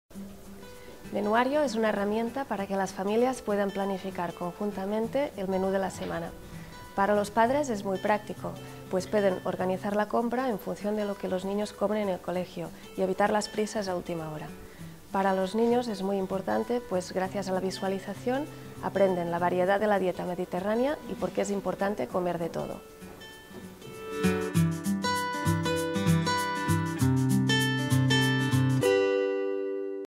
Pack Menuario. Mejora la planificación del menú de tus hijos
Si siempre tienes dudas sobre cómo hacer la planificación del menú de tus hijos, ¡ahora tienes la solución para conseguir elaborar una dieta equilibrada!

Autora: Bet Plans
Menuario, 2017
Edad recomendada: 2-12 años